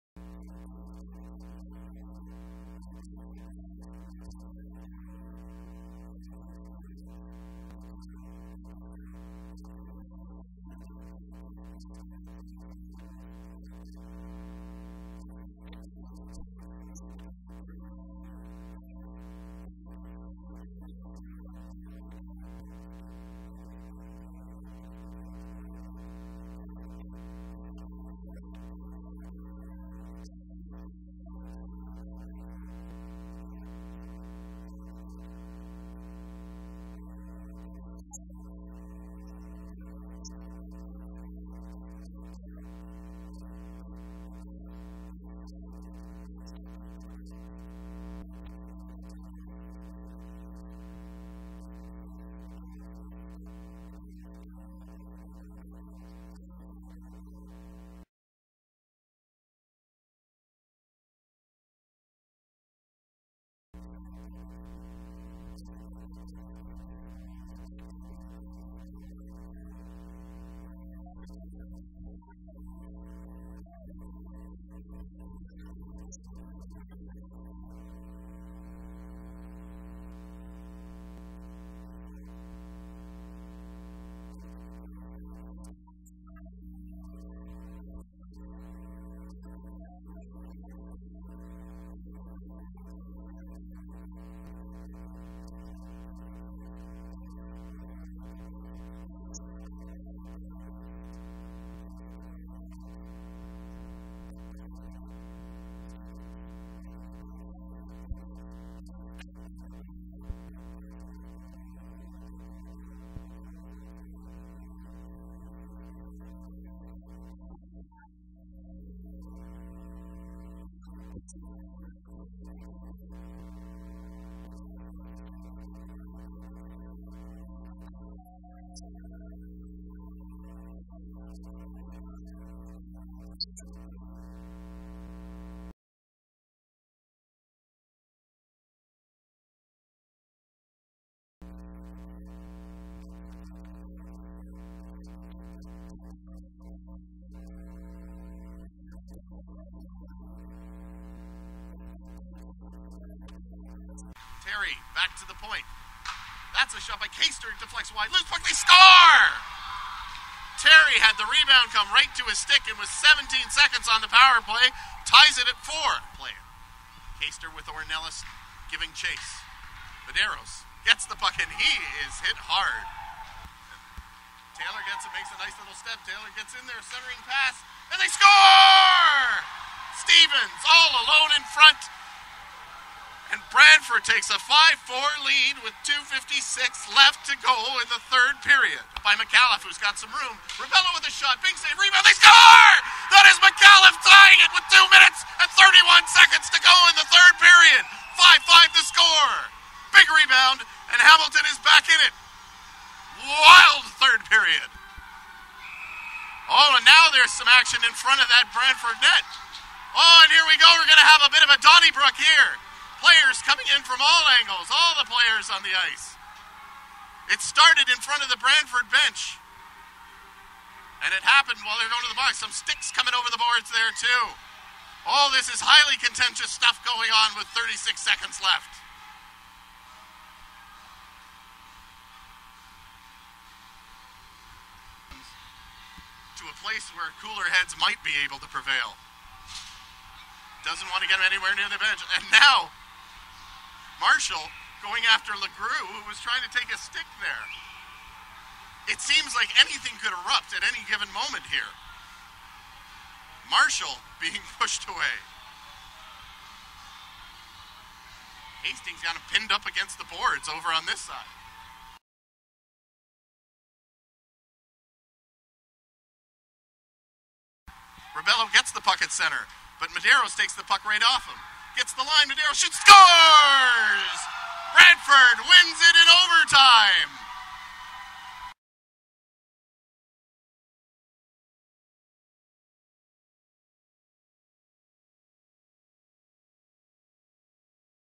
Brantford Blast at Steelhawks ACH Highlights February 11
As Hamilton battles to get into the final playoff position they took on the Brantford Blast on February 11th.  The Steelhawks would get pick up a point in the standings as the Blast took the game 6-5 in overtime.